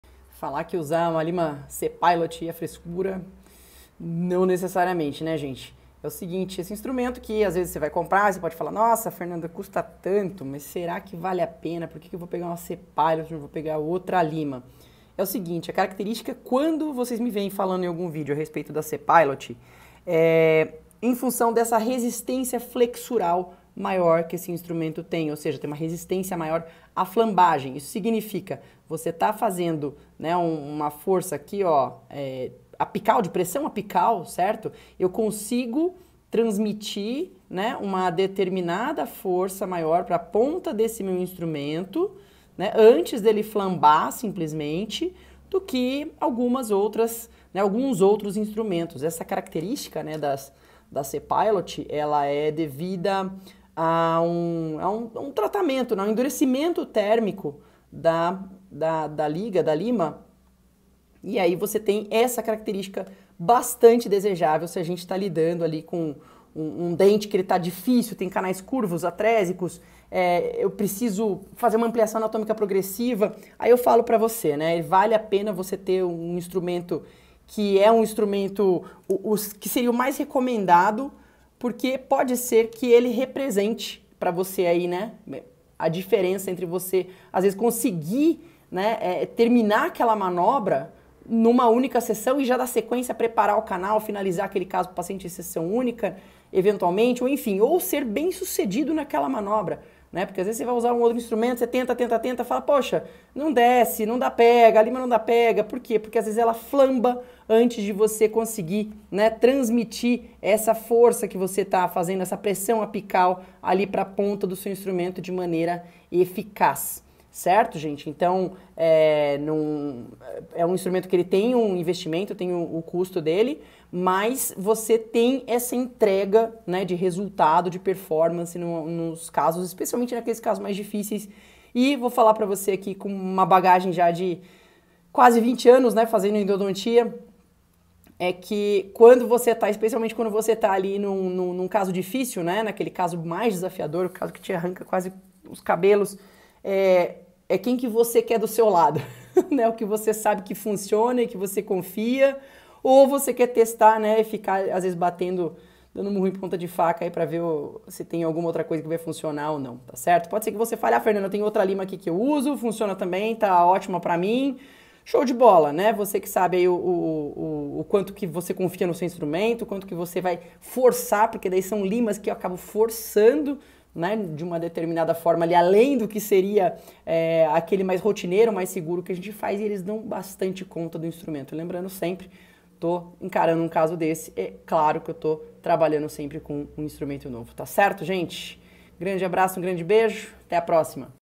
NA ENDODONTIA, ‘ECONOMIZAR’ NA COISA ERRADA PODE SAIR MUITO MAIS CARO DO QUE ESSE MATERIAL AQUI
Não é de hoje (e não é exclusividade da endodontia) a verdade por trás da premissa que o ‘barato sai caro’.

Depois de 20 anos de endodontia, dentre os muitos investimentos que temos que fazer no consultório, aqui vai um dos principais que costuma ‘se pagar’ muito bem.